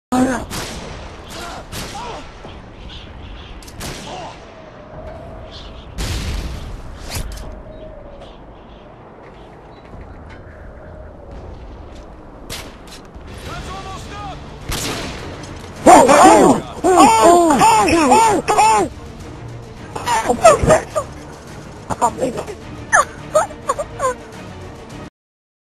insane suicide. why did you leave ?????
my 3rd day as trial in Plasma and i hit this :) that fcking fagg leaves after i killed him. he deleted his youtube after i commented . Don't ask me what i wrote on his channel :d